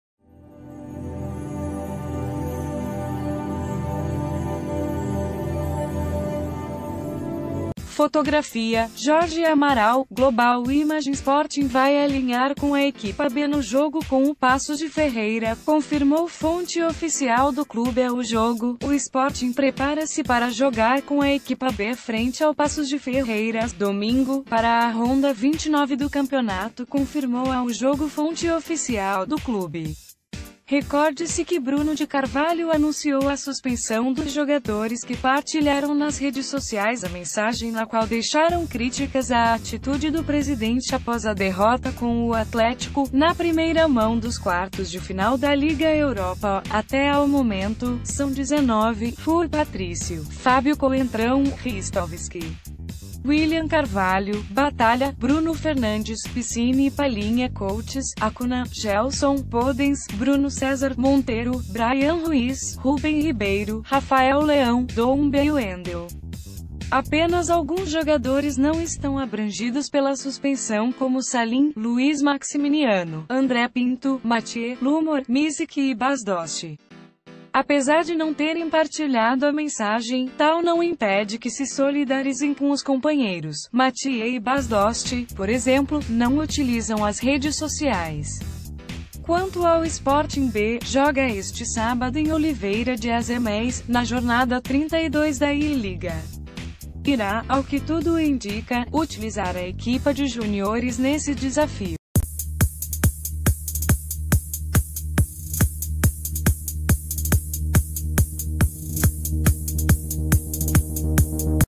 Últimas Notícias | Sporting entra em campo com a equipa B frente ao Paços
Fotografia: Jorge Amaral/Global ImagensSporting vai alinhar com a equipa B no jogo com o Paços de Ferreira, confirmou fonte oficial do clube a O JOGO.O Sporting prepara-se para jogar com a equipa B frente ao Paços de Ferreira, domingo, para a ronda 29 do campeonato, confirmou a O JOGO fonte oficial do clube. Recorde-se que Bruno de Carvalho anunciou a suspensão dos jogadores que partilharam nas redes sociais a mensagem na qual deixaram críticas à atitude do presidente após a derrota com o Atléti...